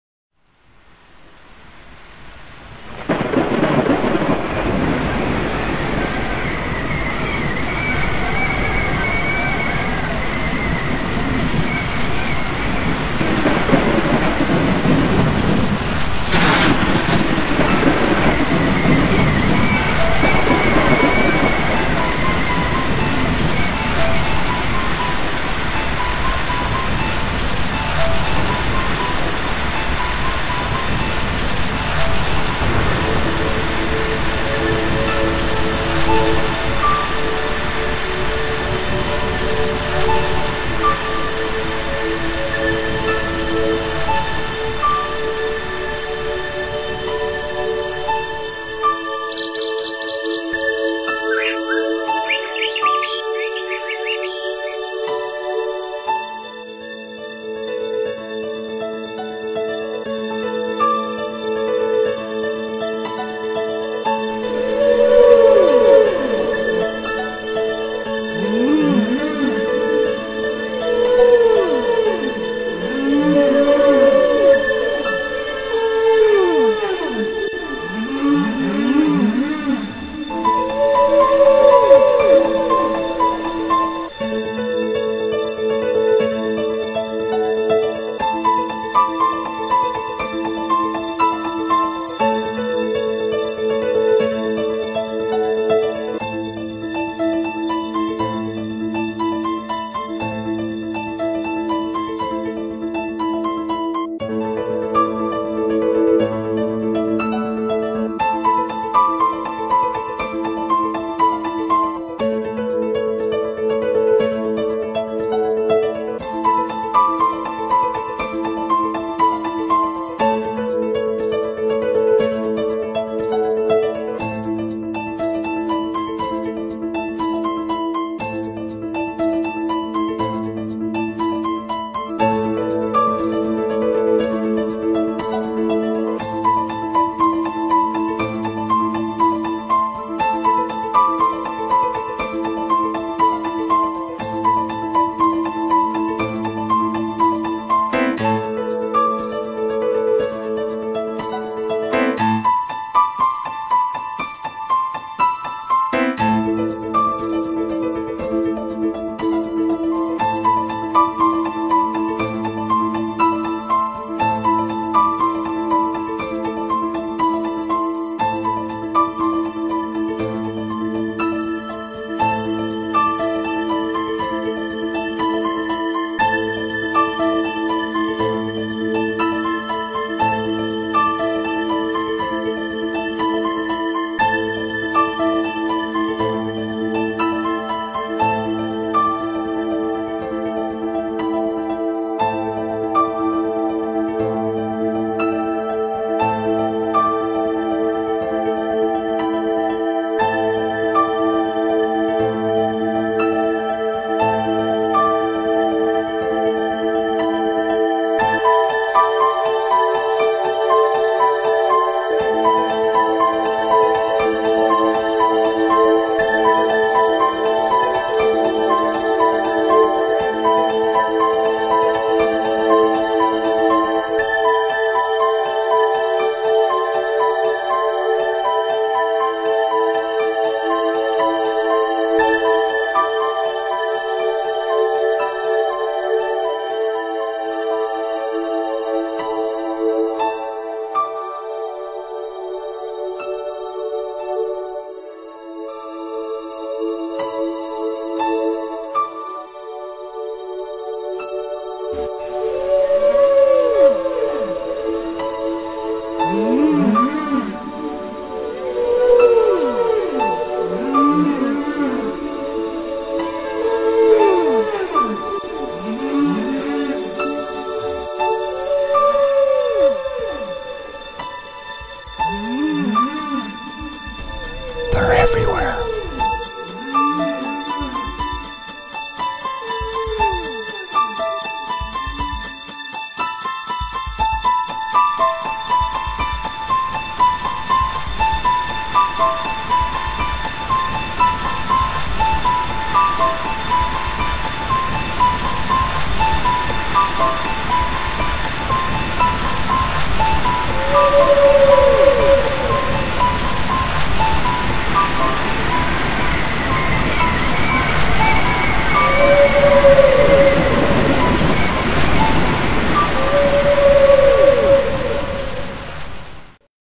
After the Storm
The video was shot after a storm at Sunderland (Roker Beach) and Seaham  UK.  Courtesy of Diademgrove. CAN YOU SEE THE WHALES

Original Music by Binladeda